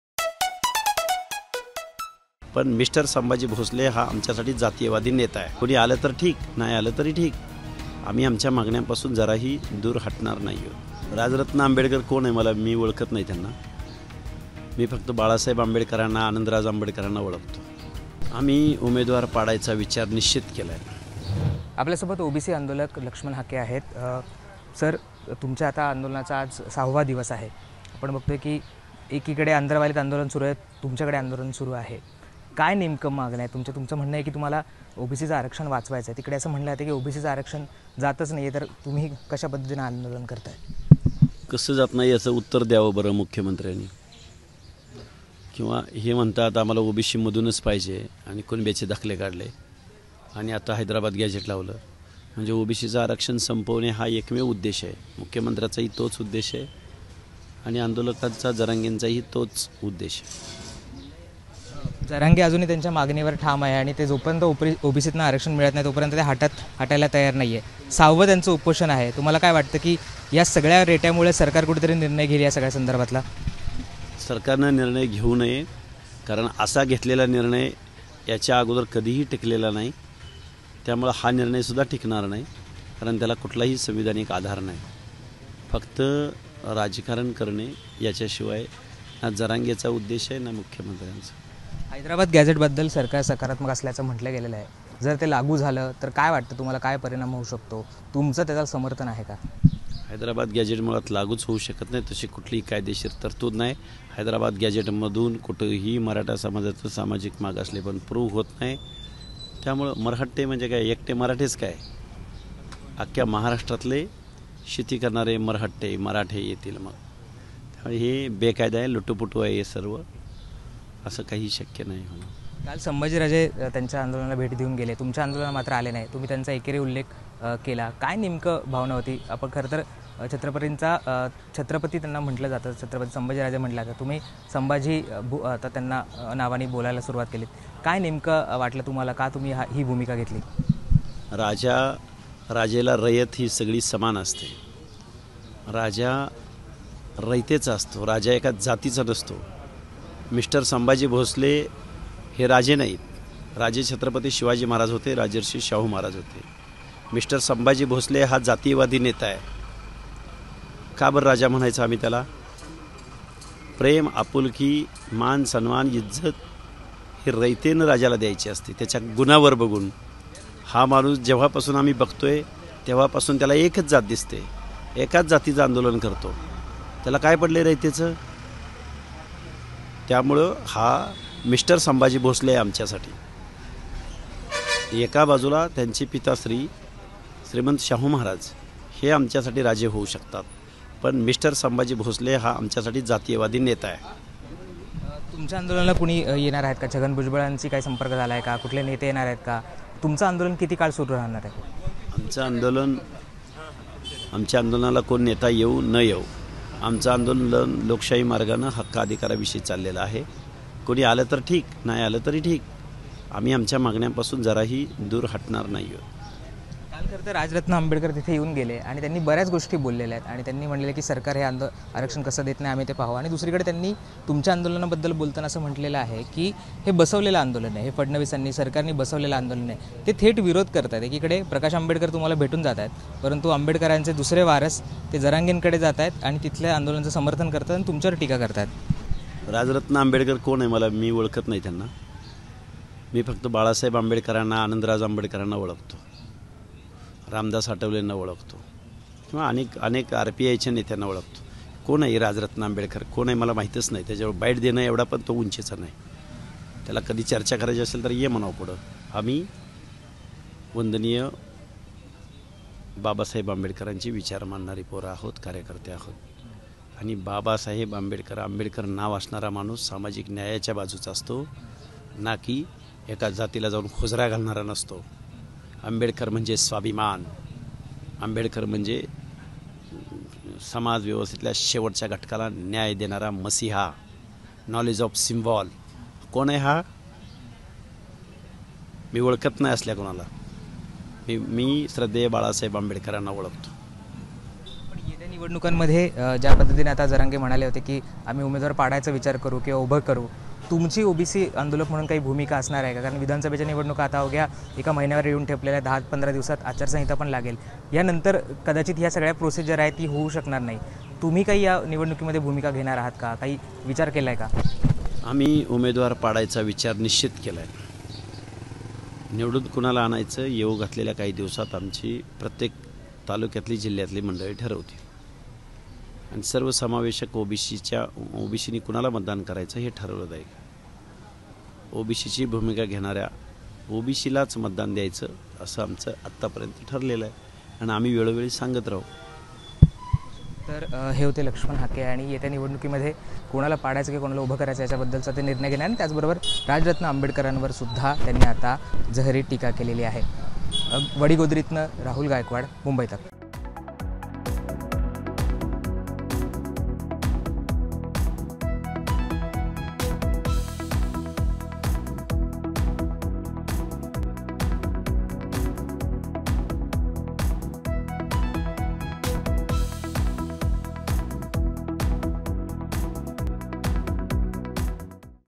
Laxam Hake Exclusive : मनोज जरांगेंचं आंदोलन ते CM एकनाथ शिंदेंना काय आवाहन? | OBC Reservation
लक्ष्मण हाके म्हणाले, “मनोज जरांगे, संभाजी भोसले किंवा अन्य कोणीही येऊ द्या, डॉ. बाबासाहेब आंबेडकरांच्या संविधानाची चौकट, त्यातील निकष पूर्ण केल्याशिवाय कोणालाही आरक्षण देता येणार नाही. राजा आता राणीच्या पोटी जन्म घेत नाही. राजा आता मतपेटीतून जन्माला येतो. त्यामुळे संभाजी भोसलेंना मी सांगू इच्छितो की आम्ही फक्त छत्रपती शिवरायांना राजा मानतो. तुम्ही असं वागणार असाल तर तुम्हाला आम्ही राजा मानायचं की नाही याबाबत रयतेला आता विचार करावा लागेल”.